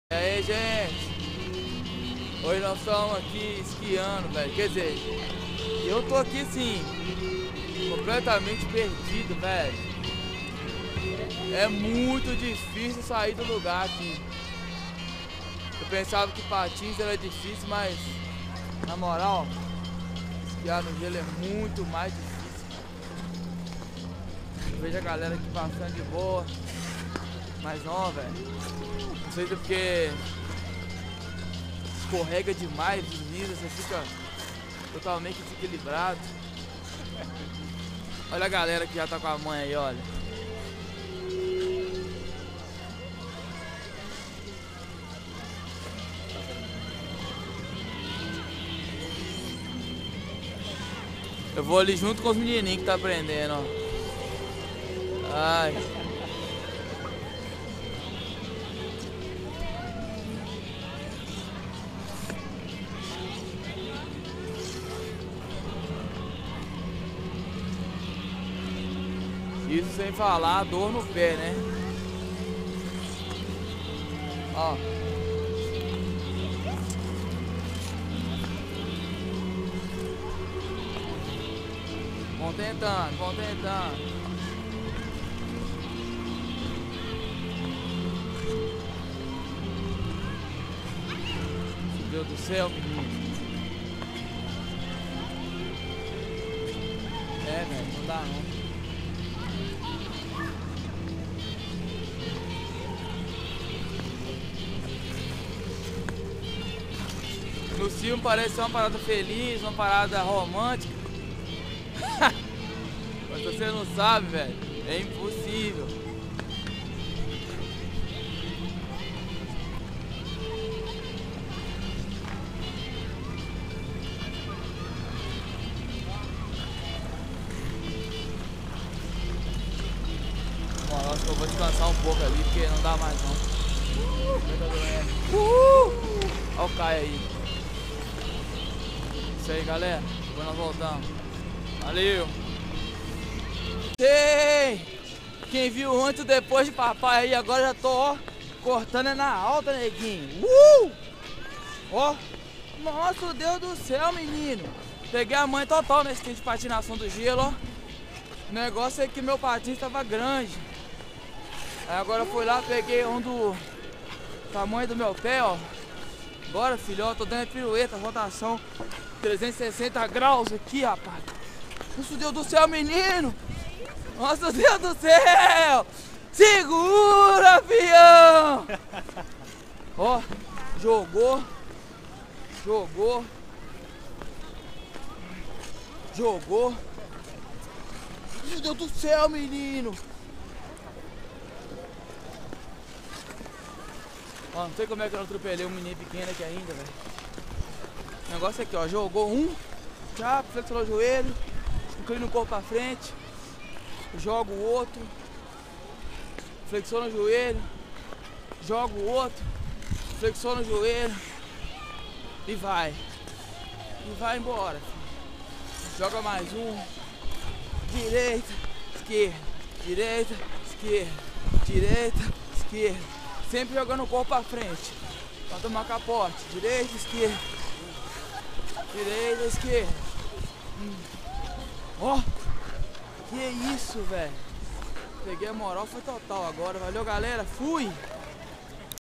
Patinando no Gelo em Dolder Suiça!!!
Patinando no Gelo na pista em Dolder na Suiça, eu faço o ante e o depois de quando eu troco meu patins hehehe porque comecei usando um patins maior que meu pe e estava muito ruim pra patinar depois eu troco eu fica beleza. O video ficou muito engraçado!!!